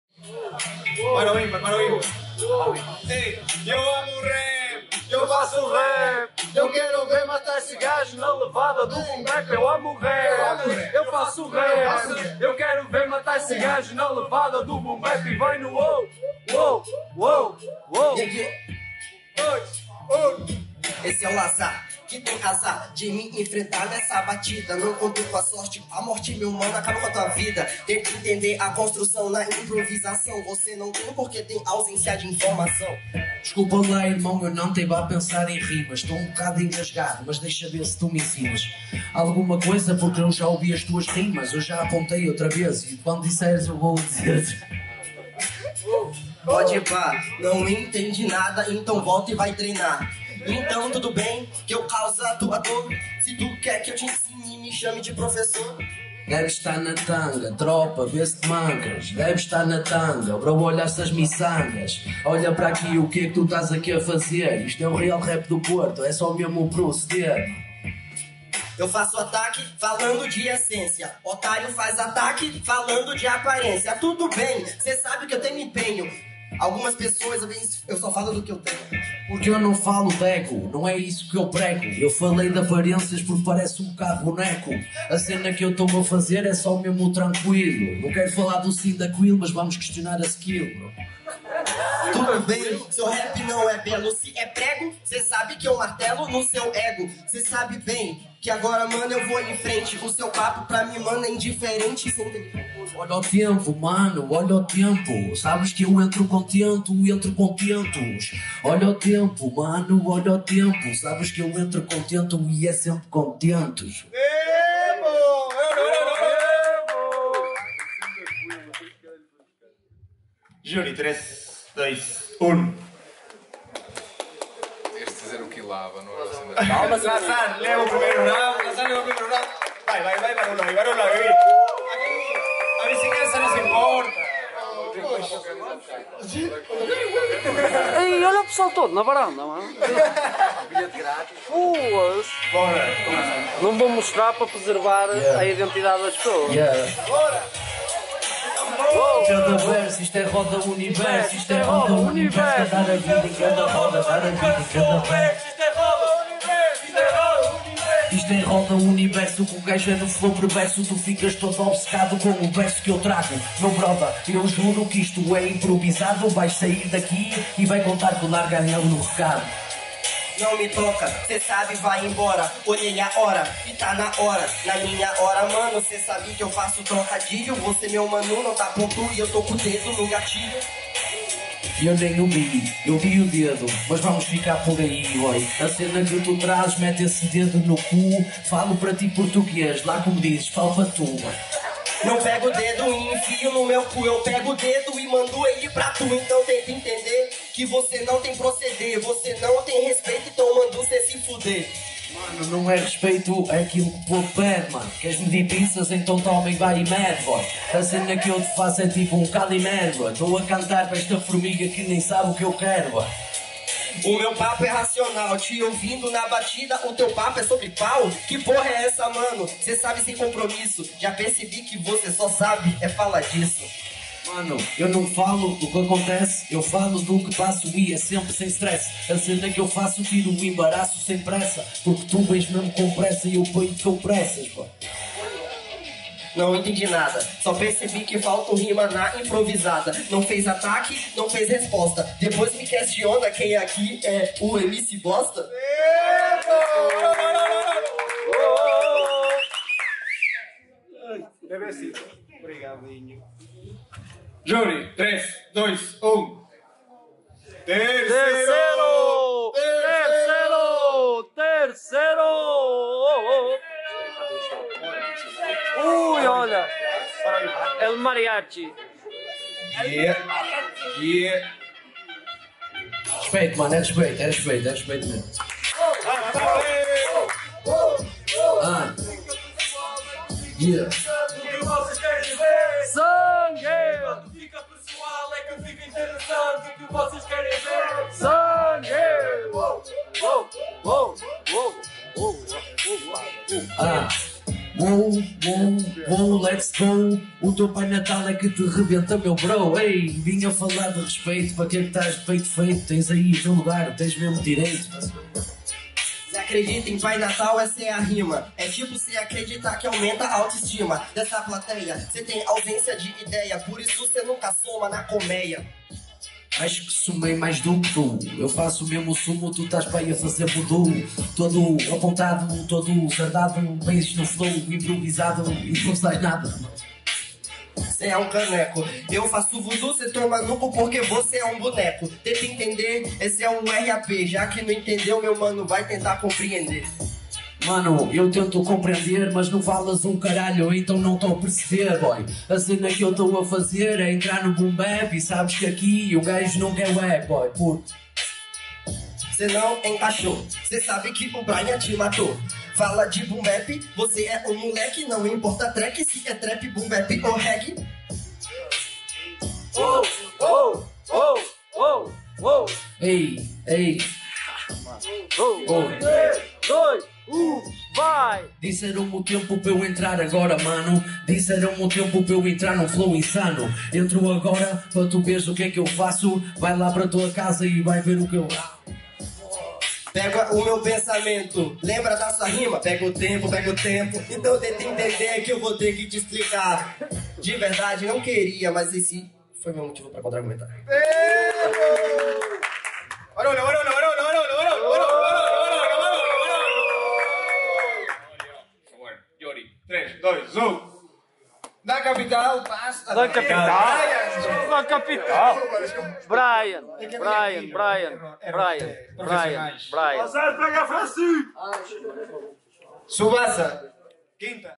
2DA FASE| LAZAR VS BRYAN | 29NA EDIÇÃO| NORTE COLOR/ FEIRA QUE FALA
Este Sábado 26 estivemos na @Norte_Color, espaço de convivio e partilha, dedicada a Cultura Hip Hop e mais intensamente ao Graffitti.

Convidados pela @Feiraquefala para promover o nosso movimento dedicado ao Freestyle em Portugal, tivemos batalhas intensas entre nossos 12 competidores e um júri de qualidade conformado por:

Agradecer a todos os que acreditam em nós!